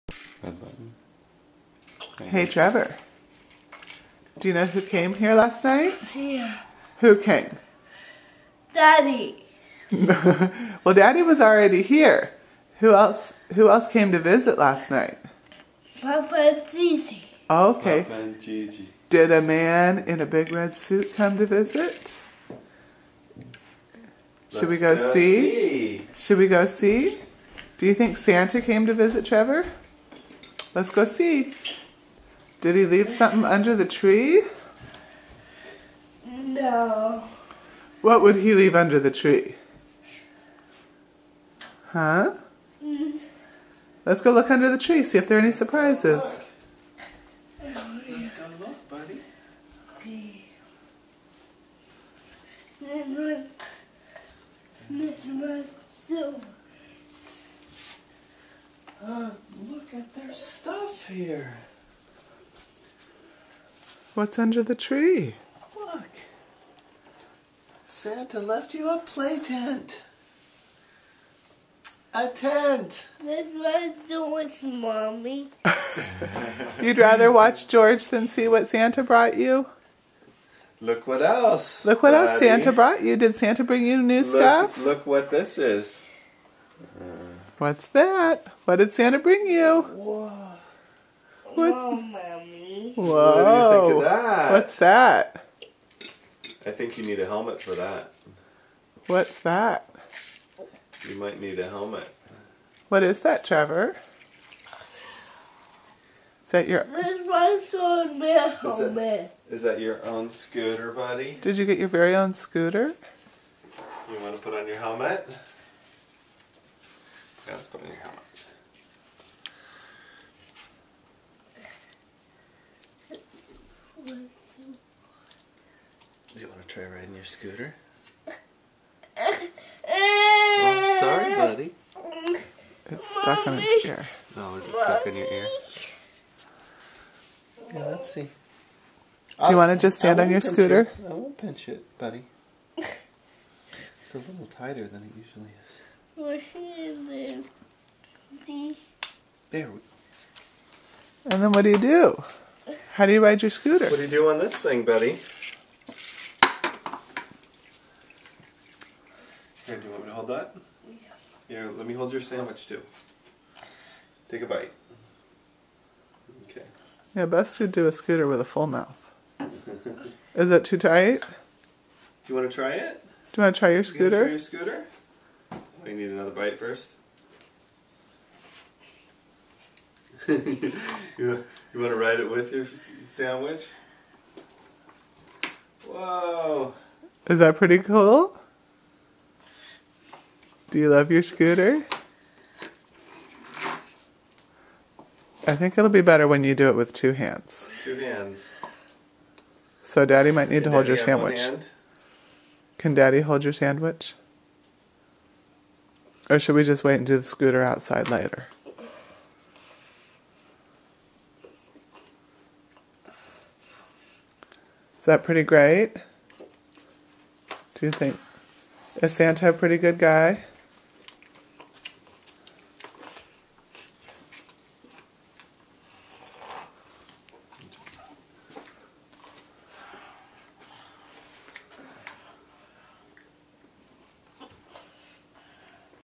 Santa_came.MP4
Okay, so the Santa concept needs some work...  First Christmas for T at home and not at a hospital.  Catching up on uploading a couple of videos.  Maybe next year.